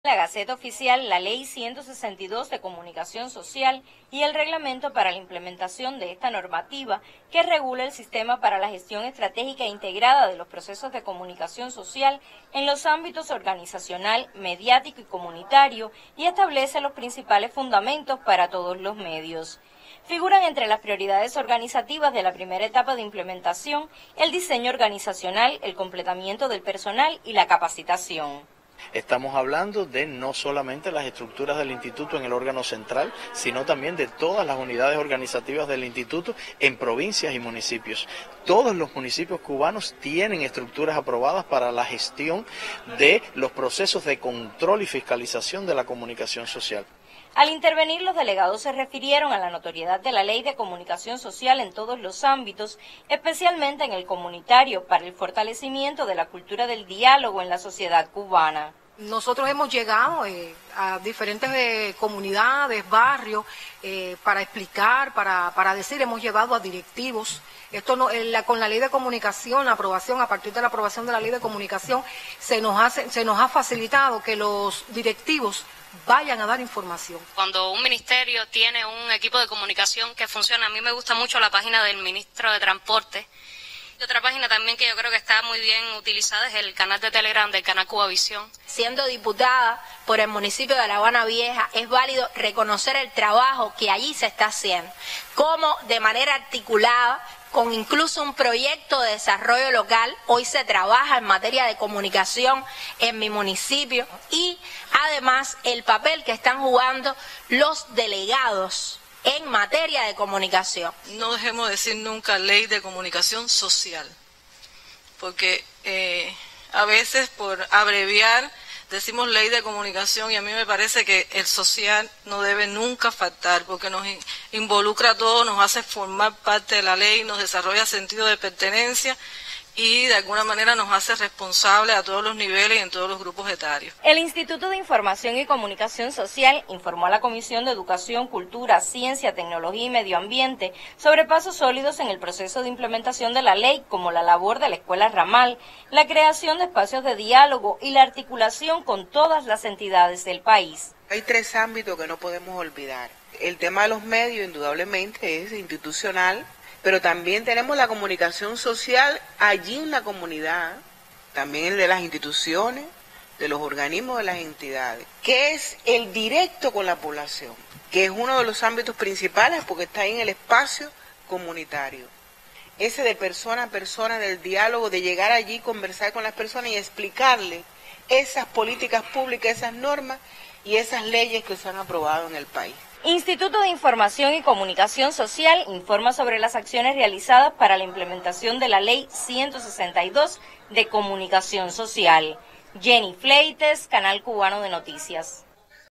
Implementación de la Ley de Comunicación Social en Cuba
El Instituto de Información y Comunicación Social presentó las acciones organizativas para la Implementación de la Ley 162 "De Comunicación Social" y su reglamento, durante un encuentro de trabajo de la Comisión de Educación, Cultura, Ciencia, Tecnología y Medioambiente.